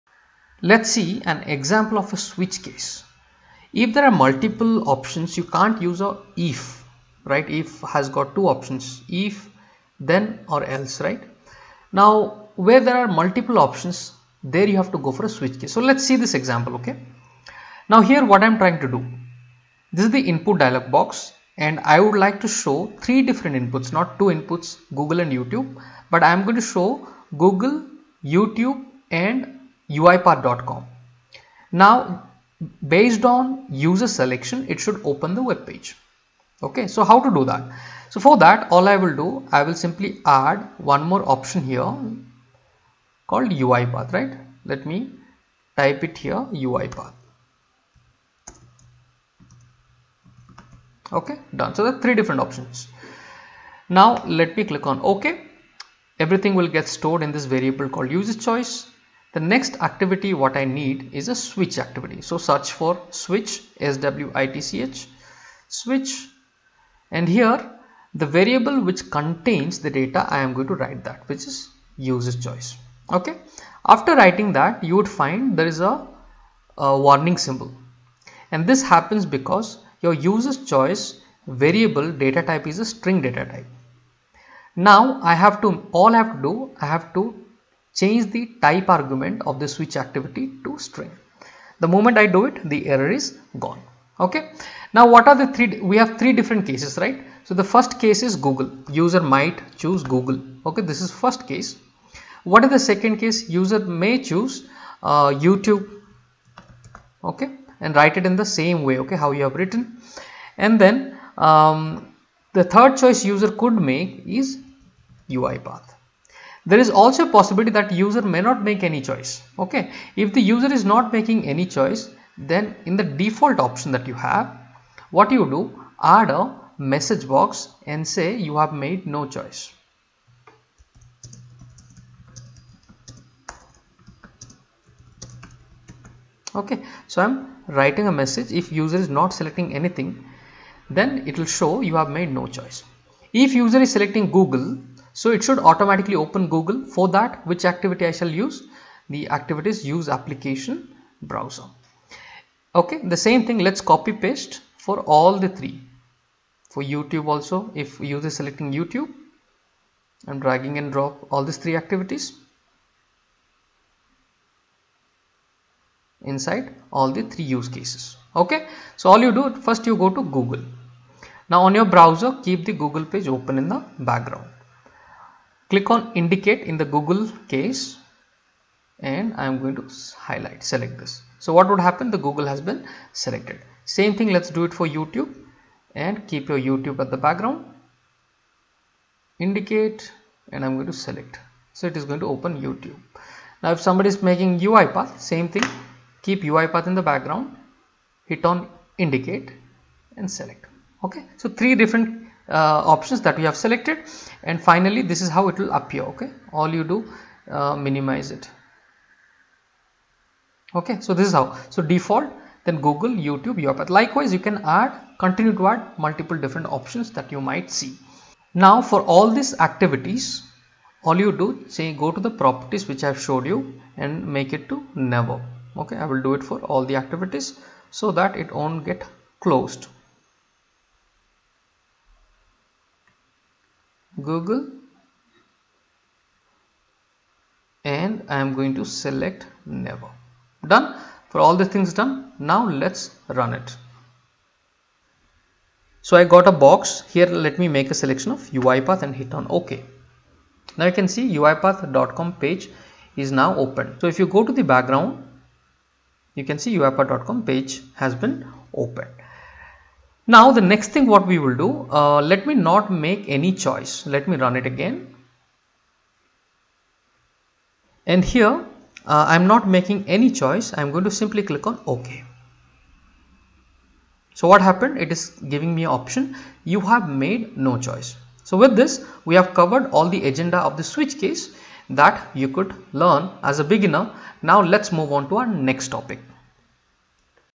HOWTO: UiPath Switch Example
Learn how to use the UiPath Switch with practical examples. Switch Uipath activity is very useful when we have multiple conditions. This uipath swithc tutorial will help you understand all that you need!